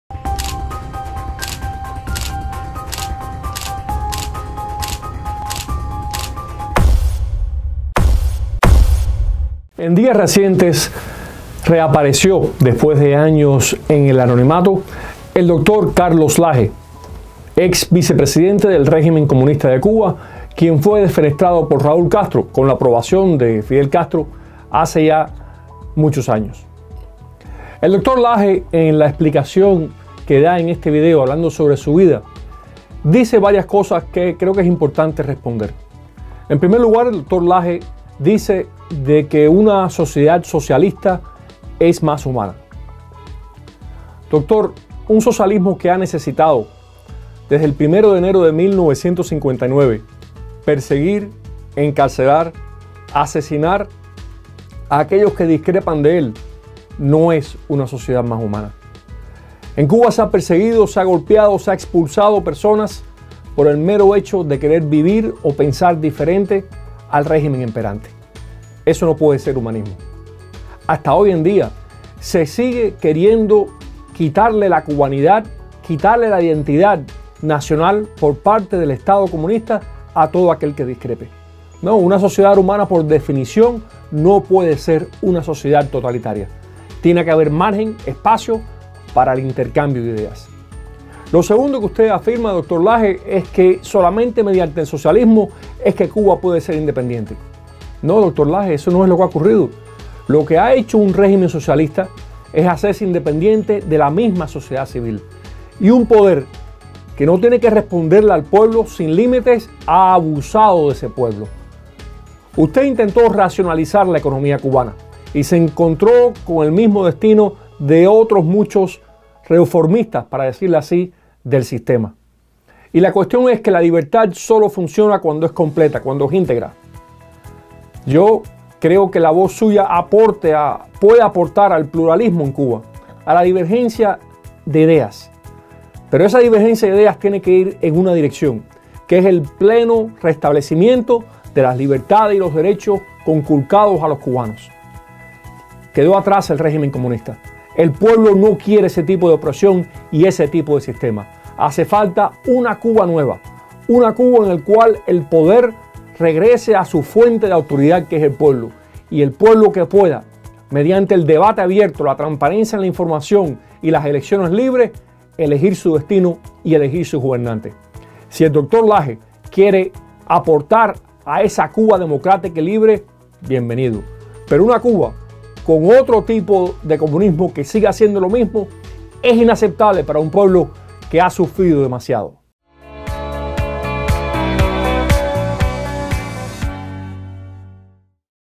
Respuestas necesarias al Dr. Carlos Lage | Revelando Cuba
El Dr. Orlando Gutiérrez-Boronat comenta sobre la reaparición del Dr. Carlos Lage en la escena pública en Cuba y responde a sus conceptos enmarcados en la idea de un socialismo que no ha sido de beneficio para el pueblo cubano.

Infórmate de esto y más todos los días